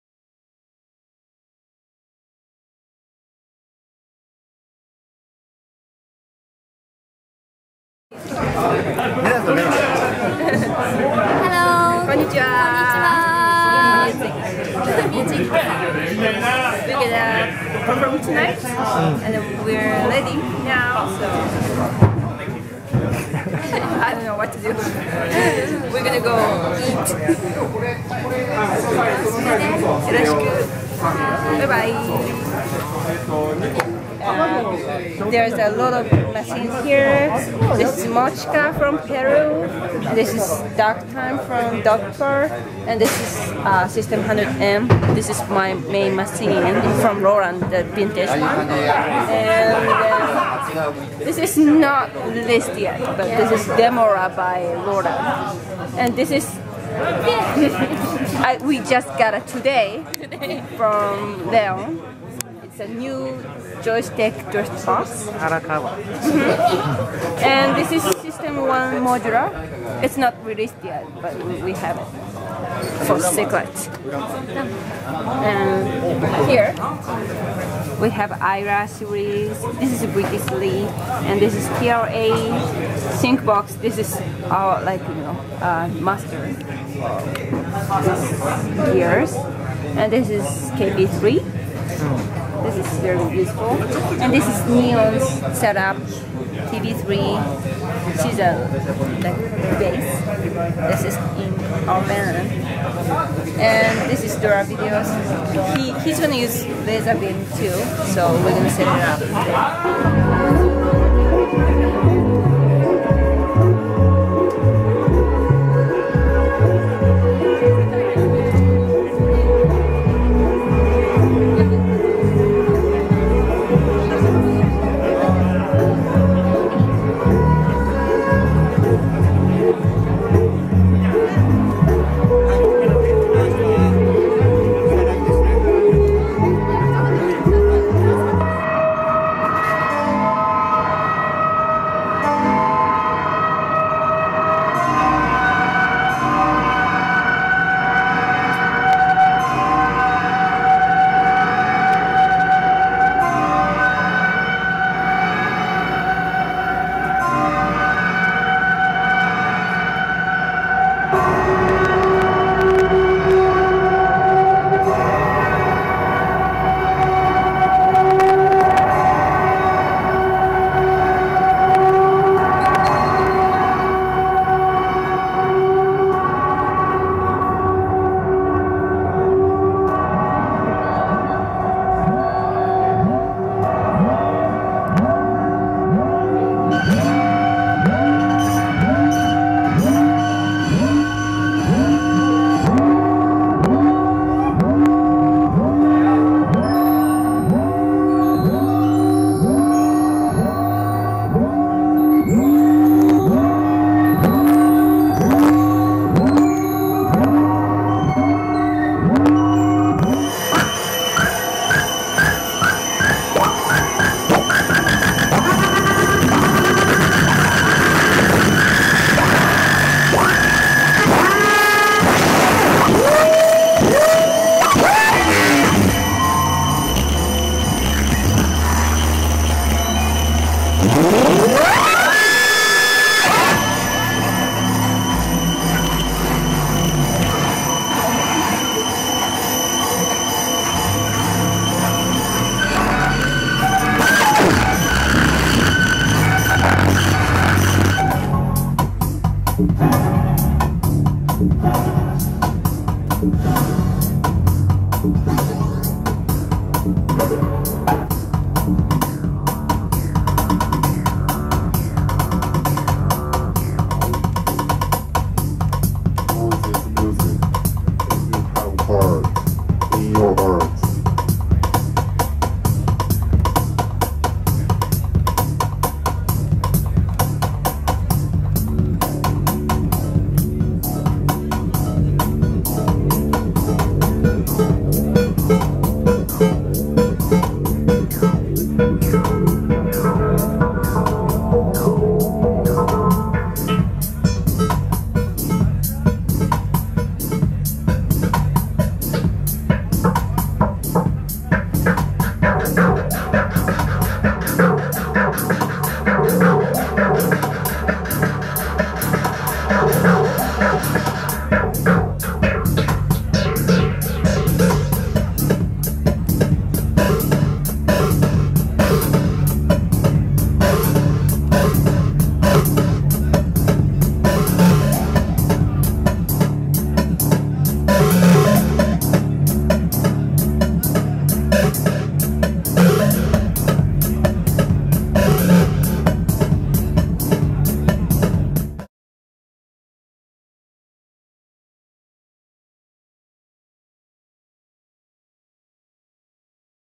Hisashi Saito+Galcid+Doravideo Live at TFoM 2015
Copyright: E.D.Music Co.
Special thanks:  Hisashi Saito, Galcid, Doravideo, Dave Skipper, Kenichi Hata